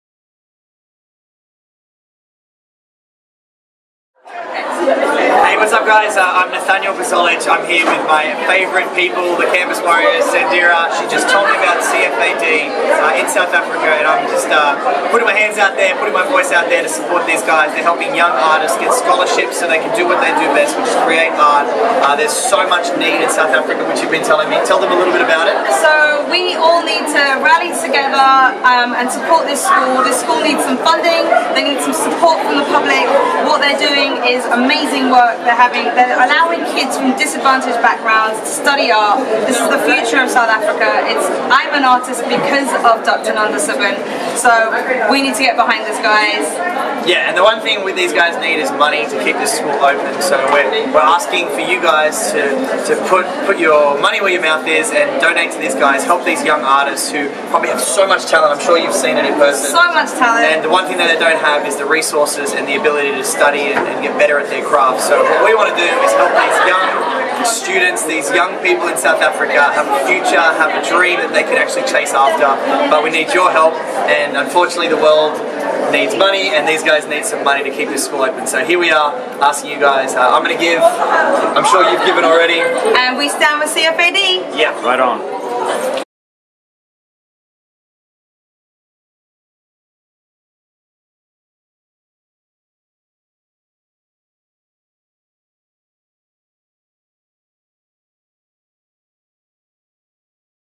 Nathaniel Buzolic & Canvas Warriors support CFAD
The talented students at the Centre for Fine Art, Animation and Design (CFAD) need your help and support!